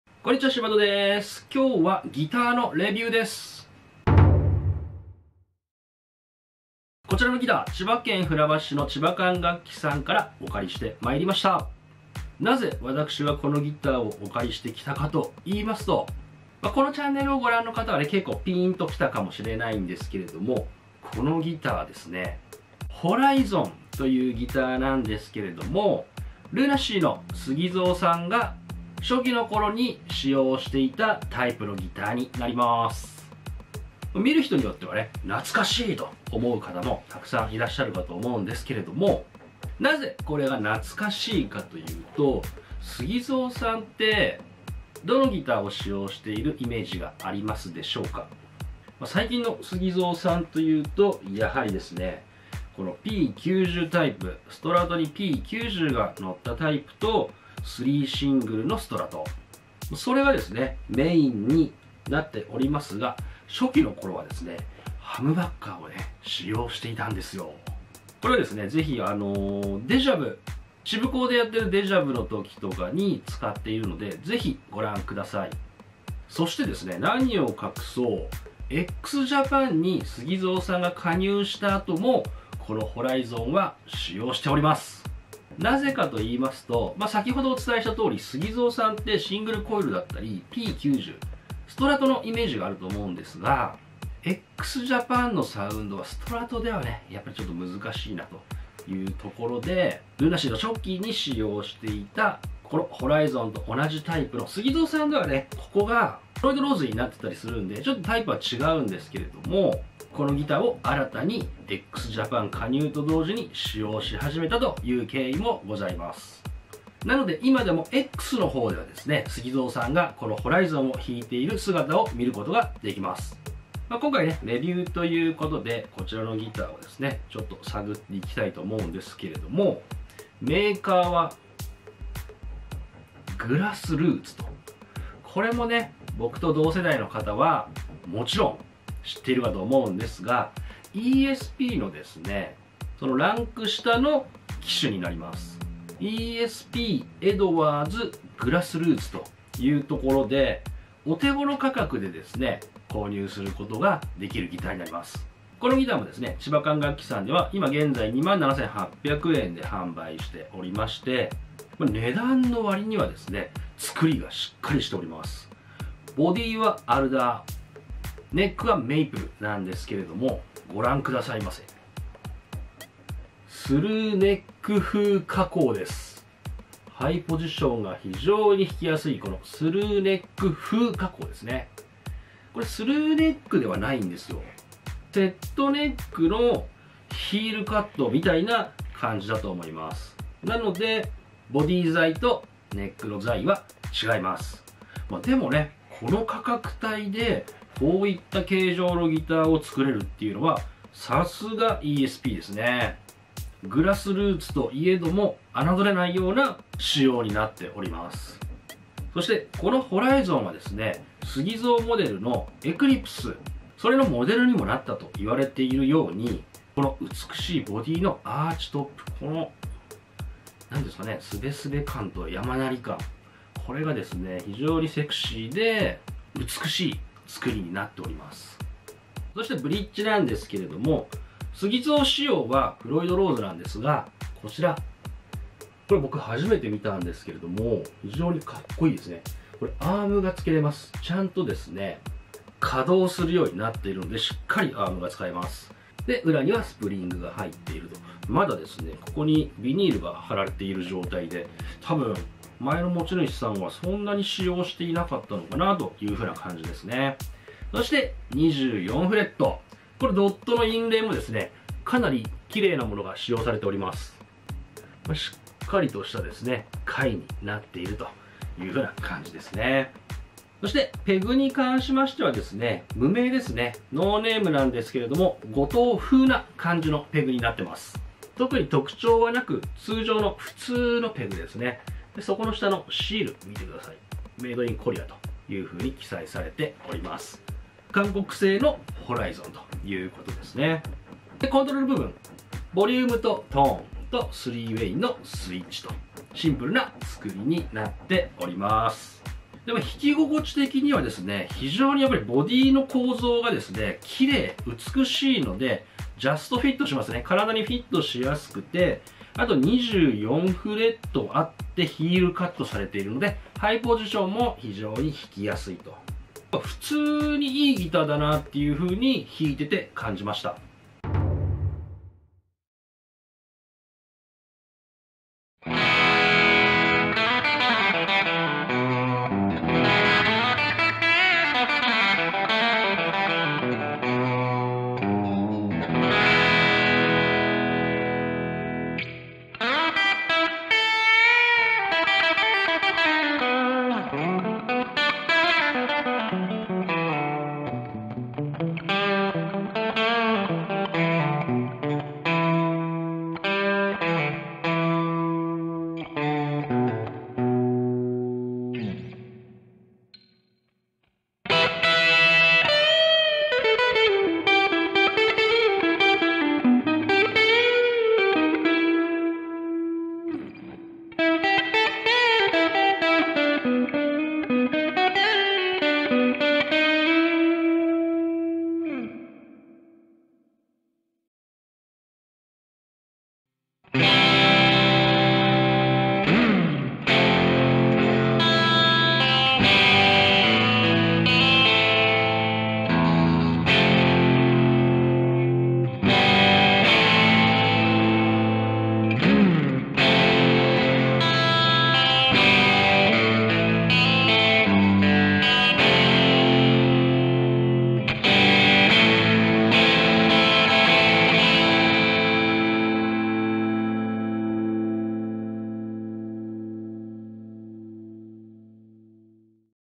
Grass Roots Horizonレビュー☆LUNA SEA初期にSUGIZOが使用していたホライゾン お値段以上のスペック！【Presented by チバカン楽器】
Grass Rootsホライゾンのレビューです！
お安いイメージのGrass Rootsですが、音はお値段以上!?
弾き心地もお値段以上!?

0:00 SUGIZOとHoraizon
2:00 Horaizonのスペック
5:22 音を鳴らしてみる

1977年生まれの私SeaBirdが、等身大の自分をお届けする新型音楽チャンネルです。